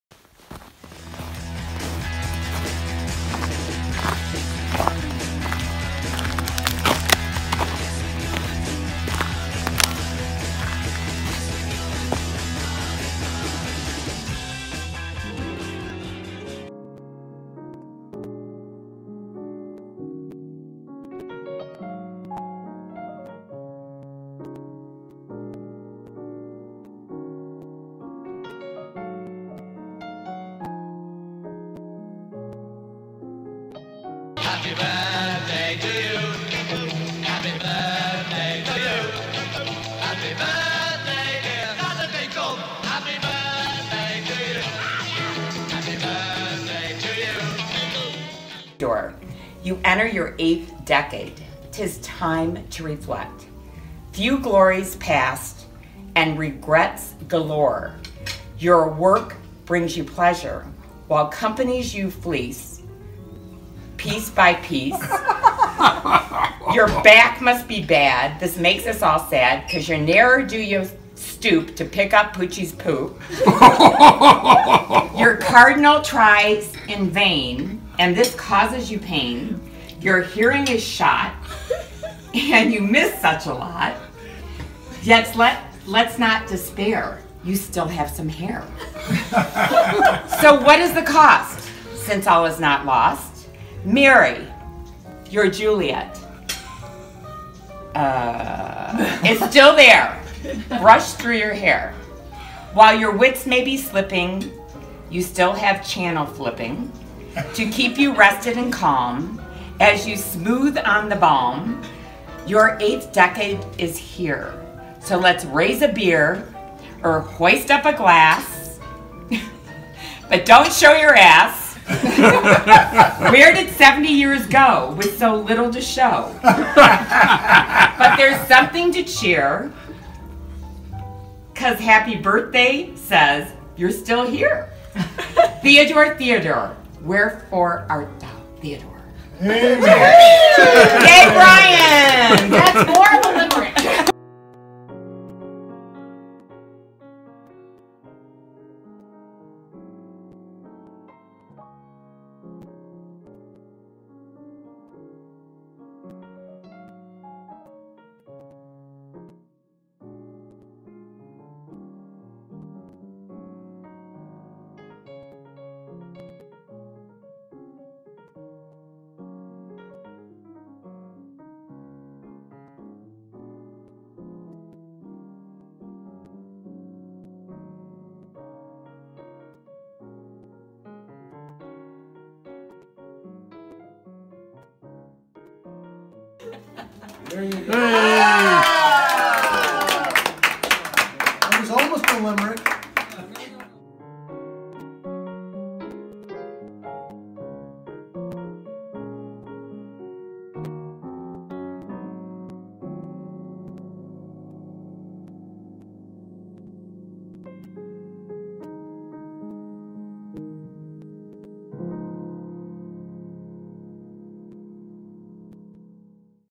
Ted's 70th Birthday
Loving father, devoted husband, sports fan, Stanford grad, and party animal celebrates his 70th birthday at dinner party hosted by his wife Mary, at Incline Village, Nevada.